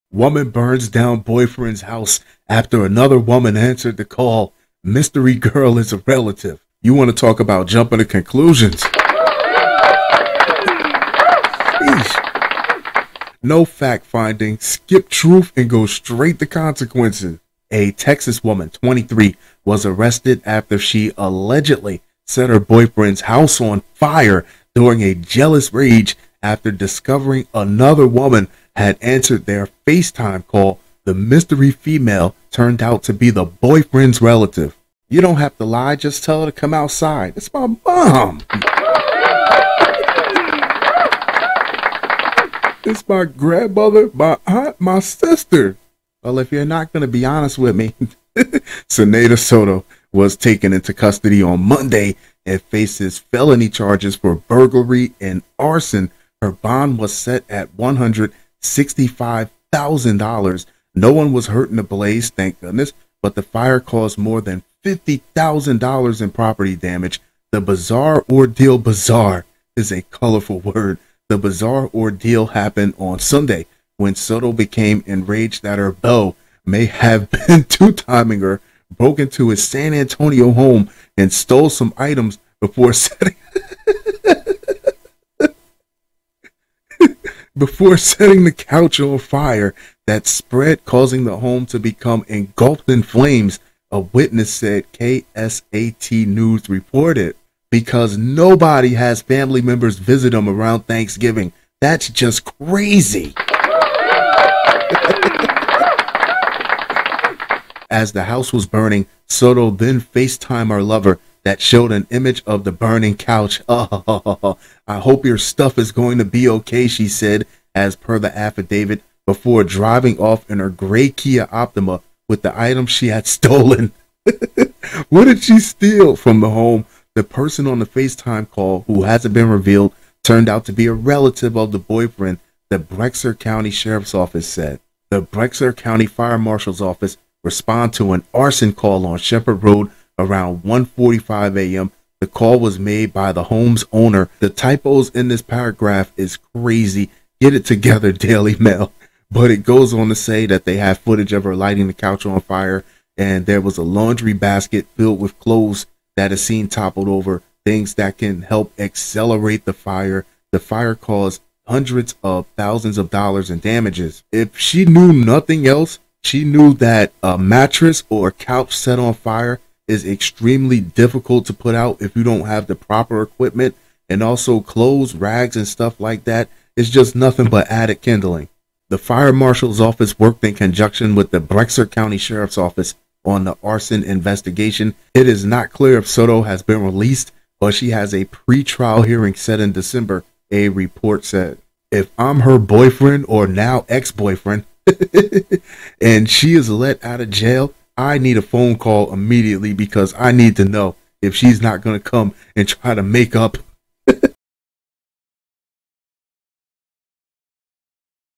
Senaida Soto Set Fire To Boyfriend's Home When Another Woman (His Relative) Answered Facetime Call
Support the podcast! Help the show grow!